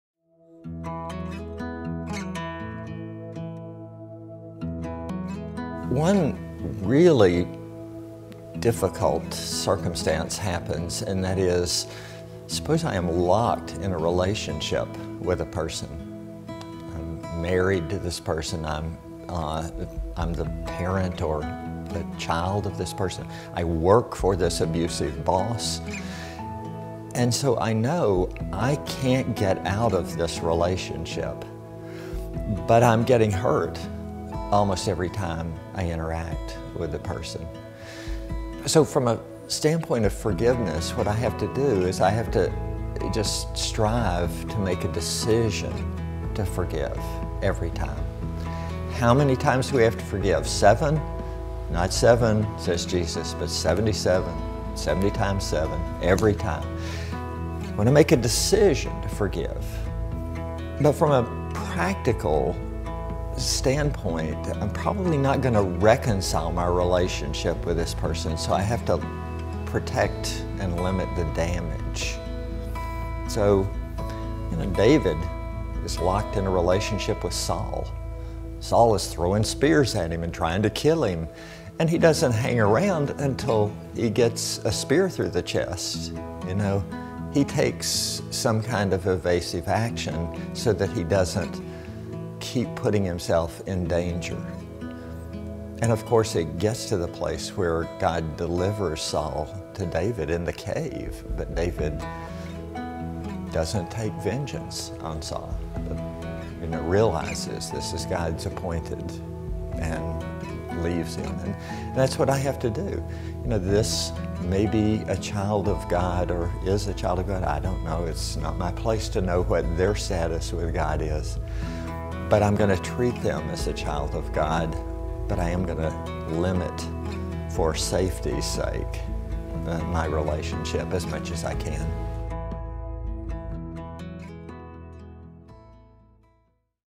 Forgiving When I'm Still Being Hurt- Dr. Worthington
How do we navigate forgiveness even when we still experience pain? Find out with Dr. Everett Worthington, a professional psychologist who has spent the last 40 years researching forgiveness, navigating both the murder of his mother and the suicide of his brother.